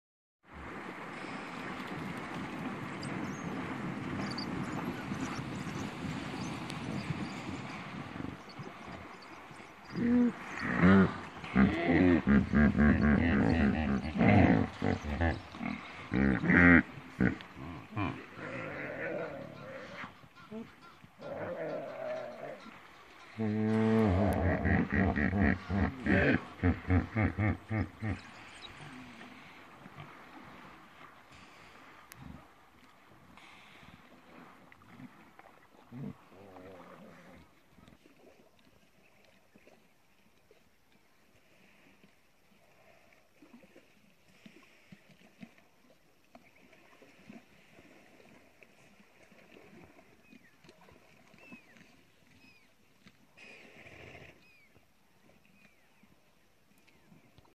Wild Dogs bring down Kudu - Savute Channel
Watch this incredible clip of a wild dog pack in pursuit of a kudu. The hippo even play a part in the action! Without a doubt the Savute Channel is producing some of the most amazing game-viewing in years.